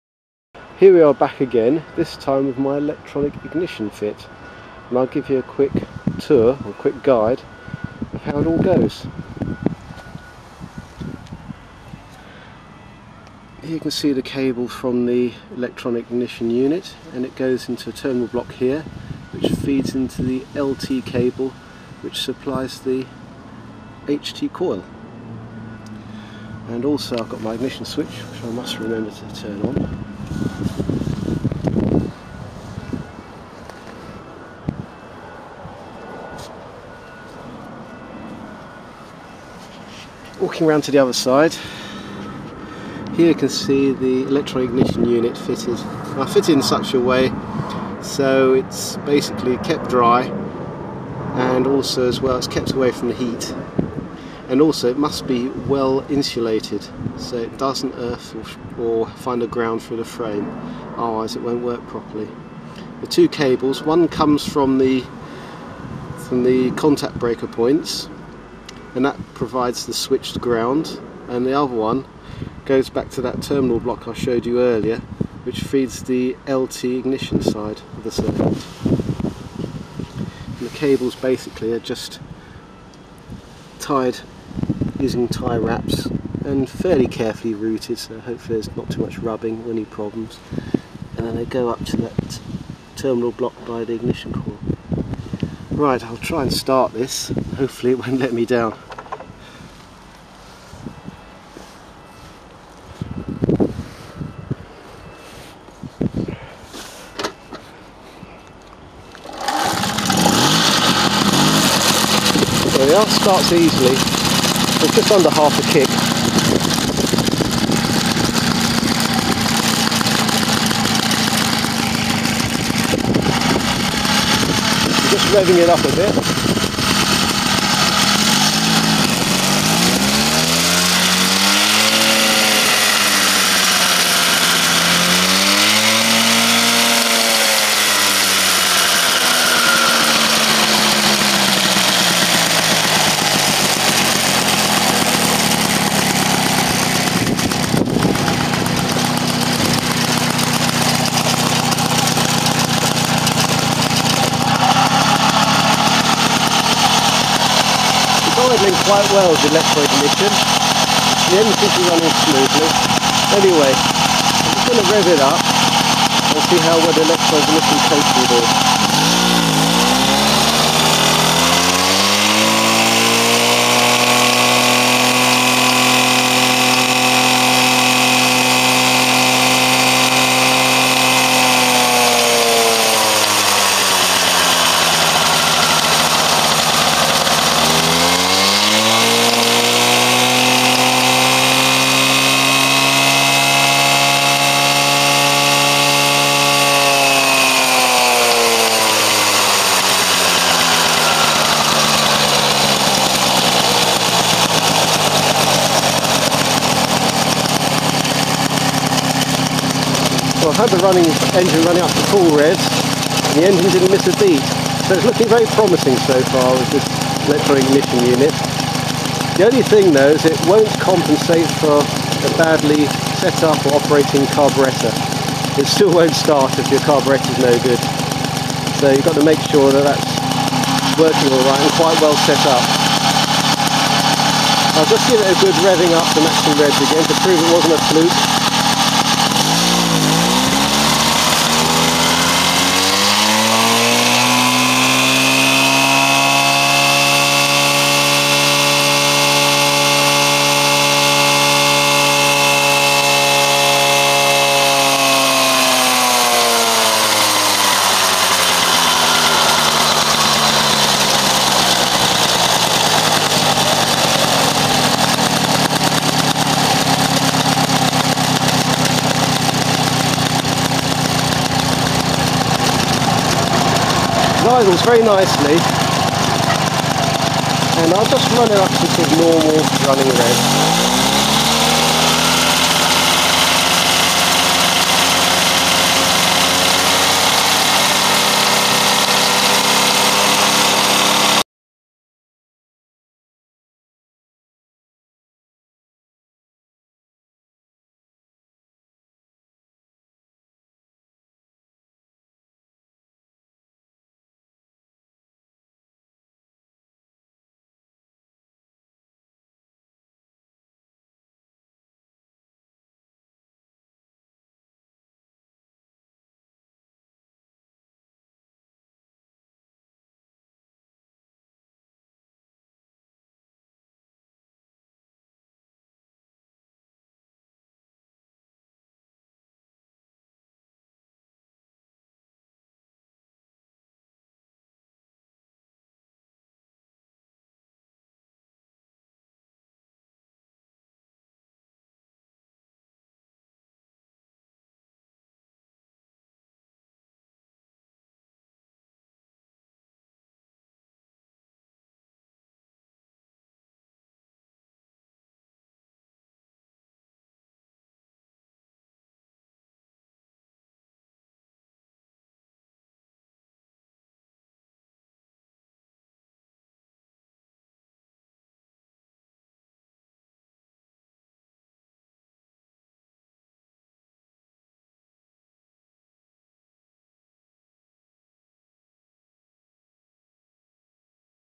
Mobylette electronic ignition experiment.
Initial test at the end of February 2014, using an electronic ignition unit similar to the Nova type, that can be used for lawnmowers, chainsaws, and agricultural equipment such as rotavators and even small tractors!